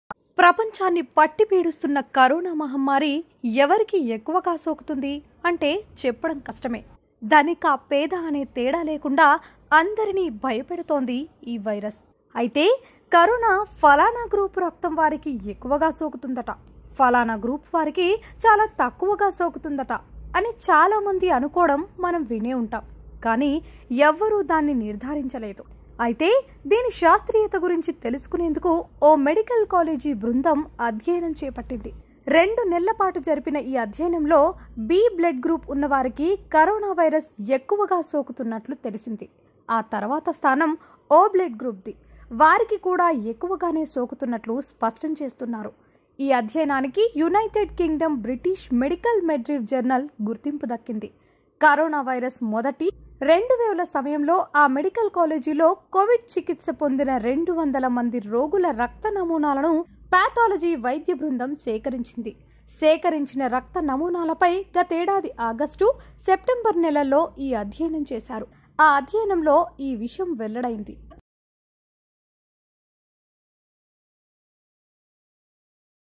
Covid-19 Insights: What Is The Role Of Blood Type In Covid-19 Infection? | Namasthe Telangana
ప్ర‌పంచాన్ని ప‌ట్టి పీడిస్తున్న క‌రోనా మ‌హ‌మ్మారి ఎవ‌రికీ ఎక్కువ‌గా సోకుతుంది అంటే చెప్ప‌డం క‌ష్ట‌మే. ధ‌నిక‌, పేద అనే బేధం లేకుండా అంద‌రినీ భ‌య‌పెడుతోంది ఈ వైర‌స్.